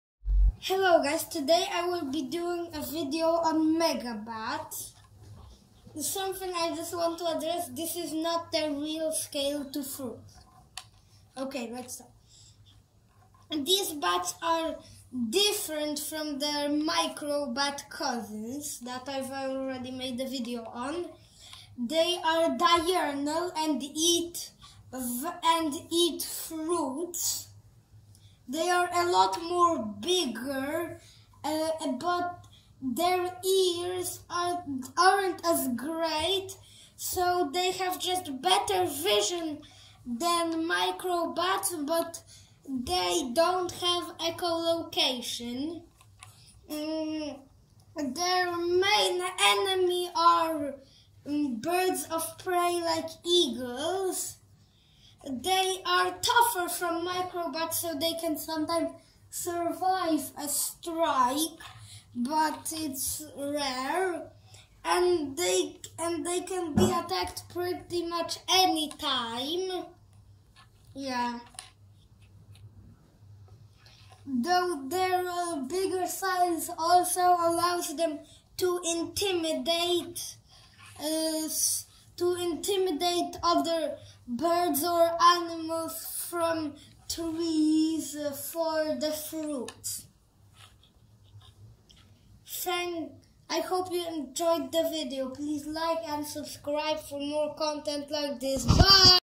Megabats - also called fruit bats
The megabats are the largest bat species, some of them are diurnal and most are unable to echolocate, relying instead on the sense of sight.
Please like and sybscribe for more content like this!